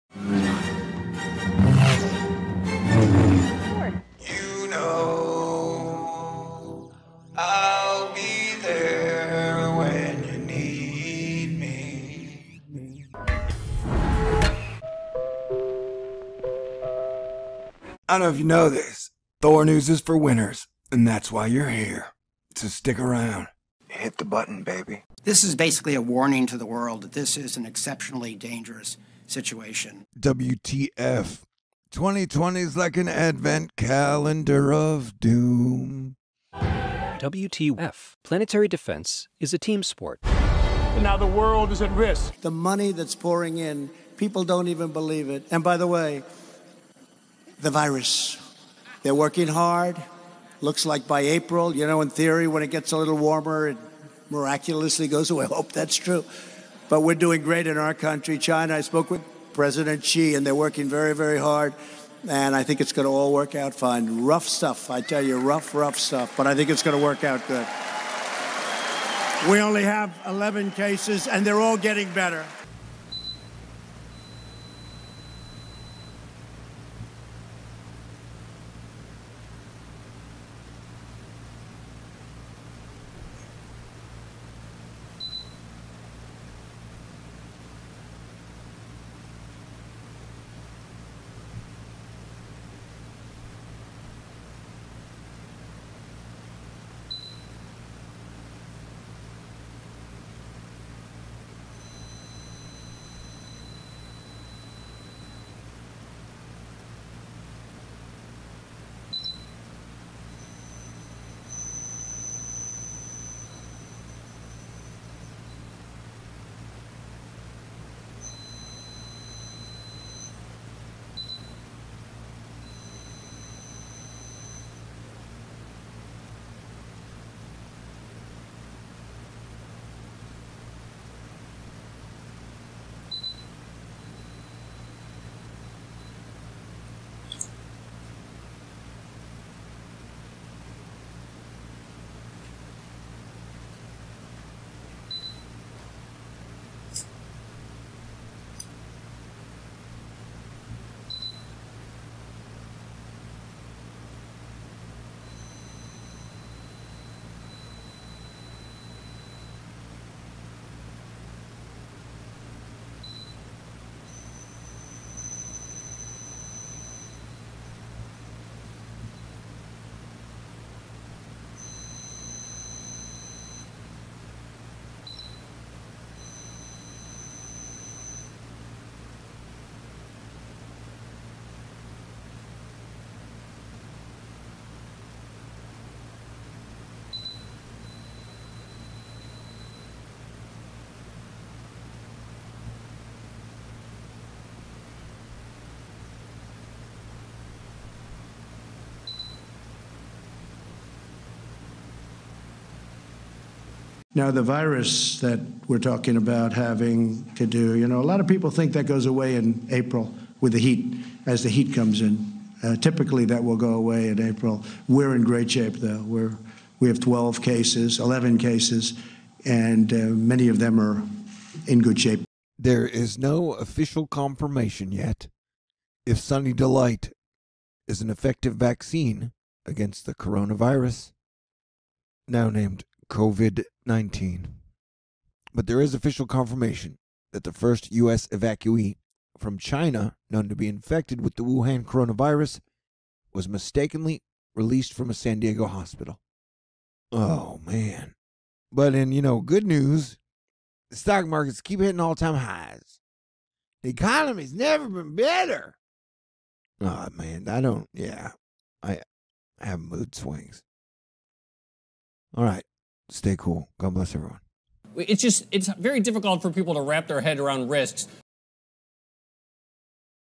Corona Virus Update: Trump response, San Diego Problem & Chinese Children
This situation is bad getting worse and Disaster Response may be our President's BIG Achilles Heel. oh it also has an official name, CoVid-19.
T LEWISON
5430 BIRDWOOD RD. #416
HOUSTON TEXAS 77096